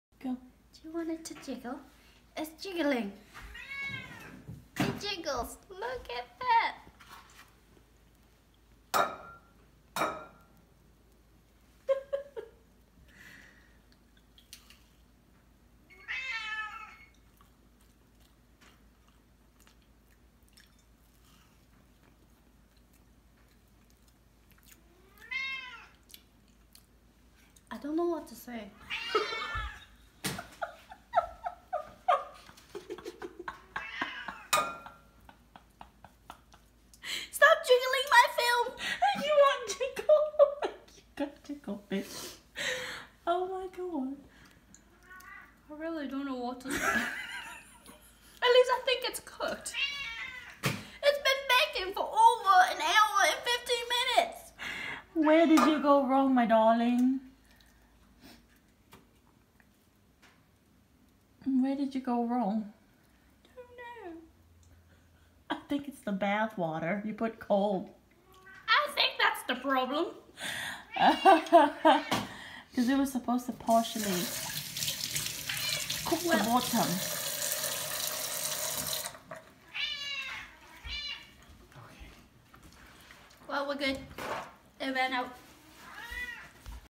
Japanese cake gone wrong !
Lesson number 1 , use hot water in the pan not cold 🤦🏻‍♀️. So um my cake didn’t rise like it should and it undercooked .. sigh !